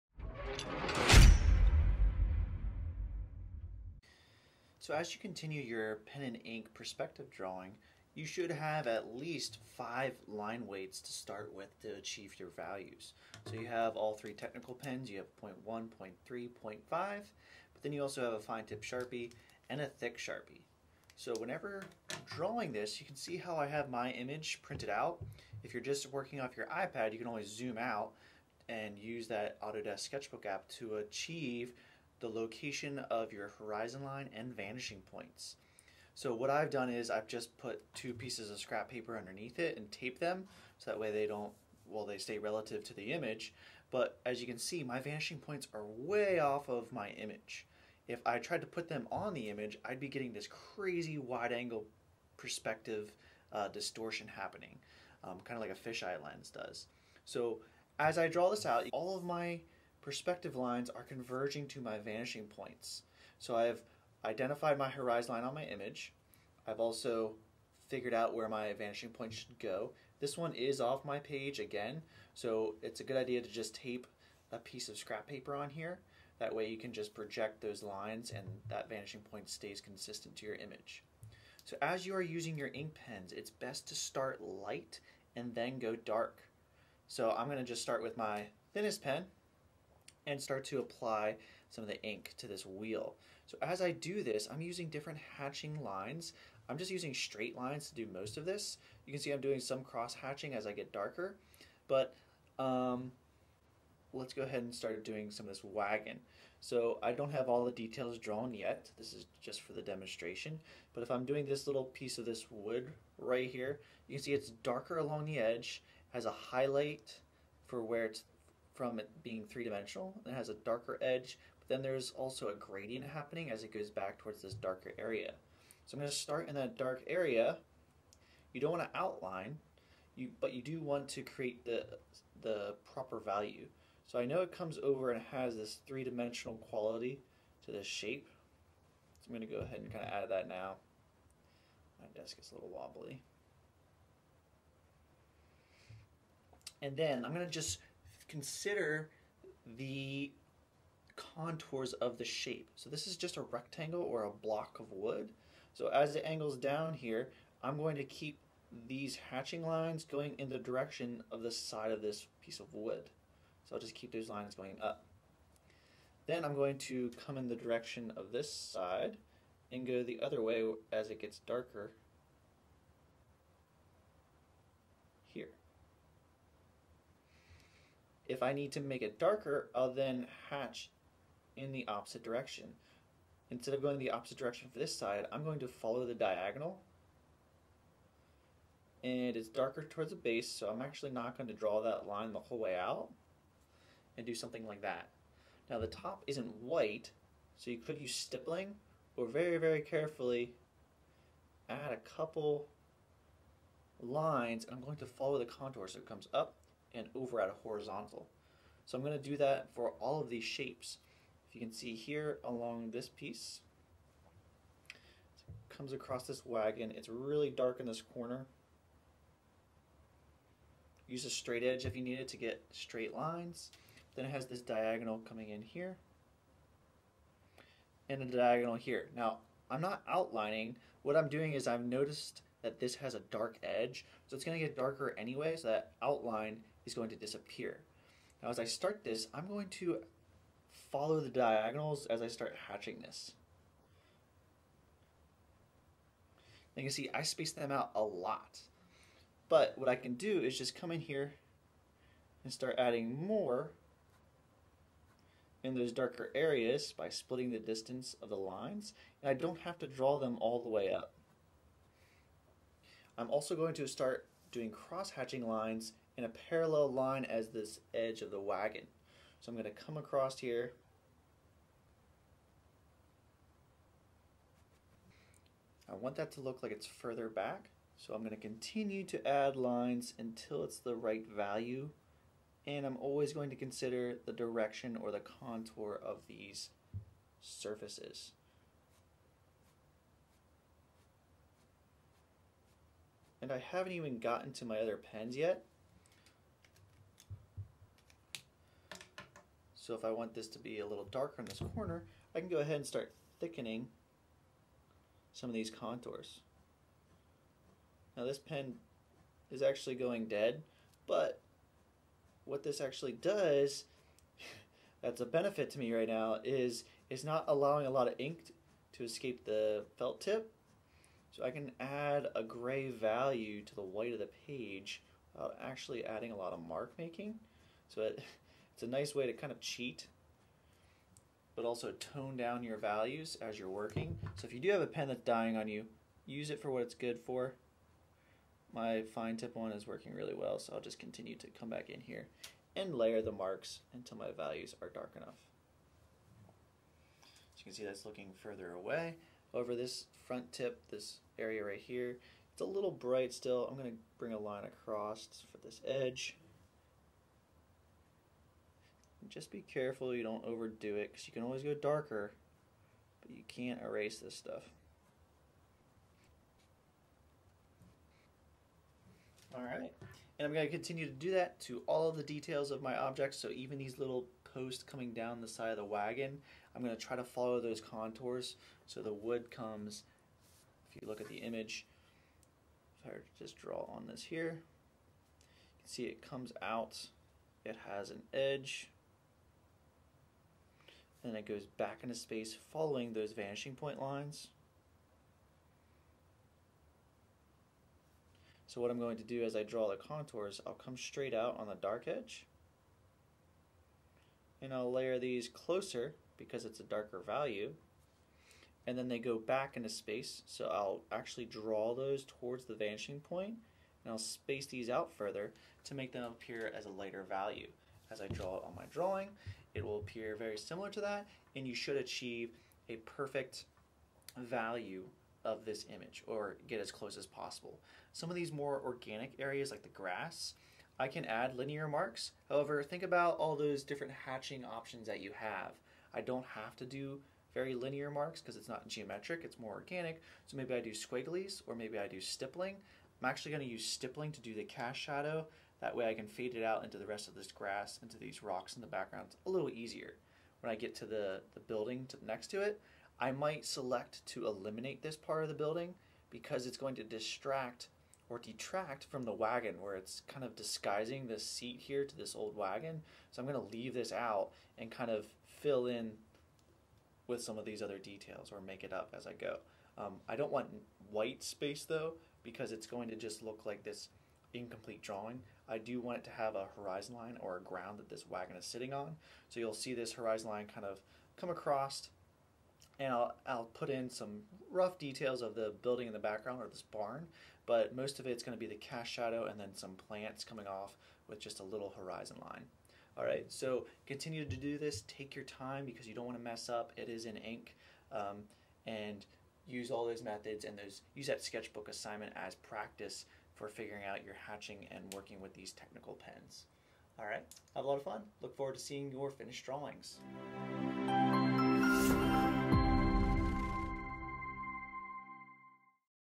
Ink Perspective Progress
This video is about the  Ink Perspective Drawing assignment.